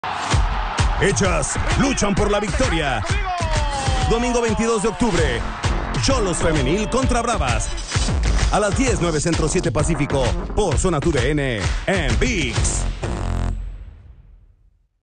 Tijuana Femenil vs Juarez Femenil | Promo | Zona TUDN en VIX 🇺🇸
Creditos a @tudn_usa y @vix

Liga MX Femenil: Tijuana vs FC Juarez
Domingo 22 de Octubre
10P/9C/7PAC
Por Zona TUDN en VIX.